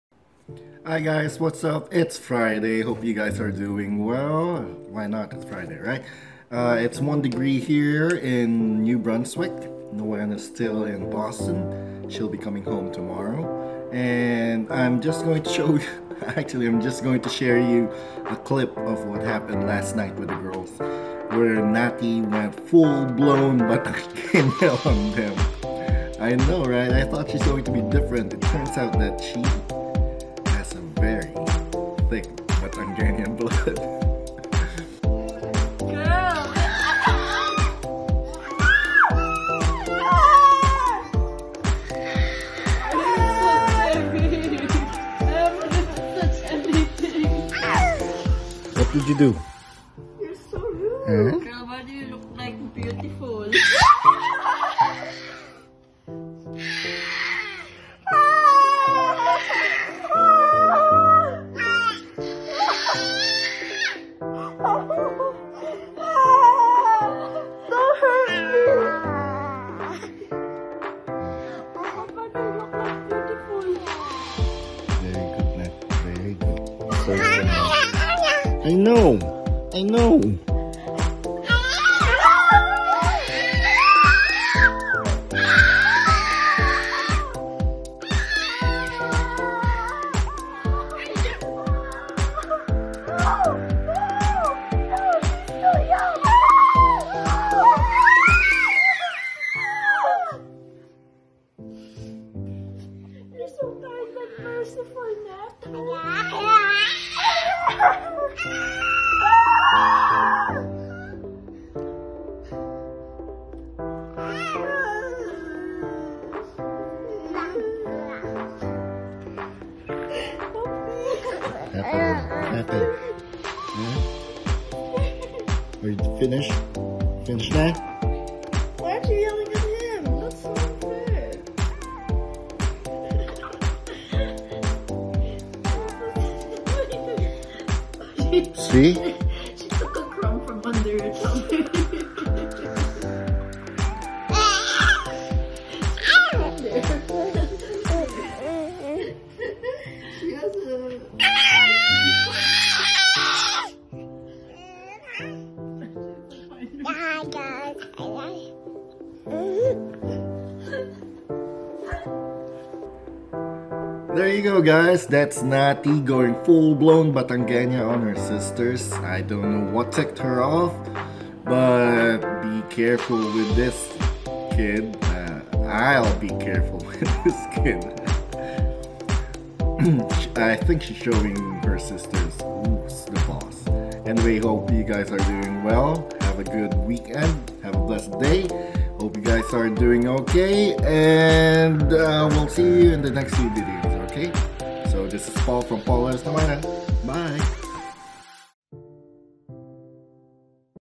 Nati's Batangueña Mode On
Hi guys!

Hope you're all doing great! It's Friday and I'm sure you're looking forward for a wonderful weekend.

Just thought of sharing this. Nati went full on Batangueña with her sisters last night. She was just up and screaming at them for who knows what.

I know they're half Batangueñas. I just didn't know their Batangueña blood is that thick, especially on this little one.  I have to be careful with this one while she's still on my side.

Anyways, hope you guys have a great weekend. We'll catch up in the next few videos. Keep safe.

Bye 😀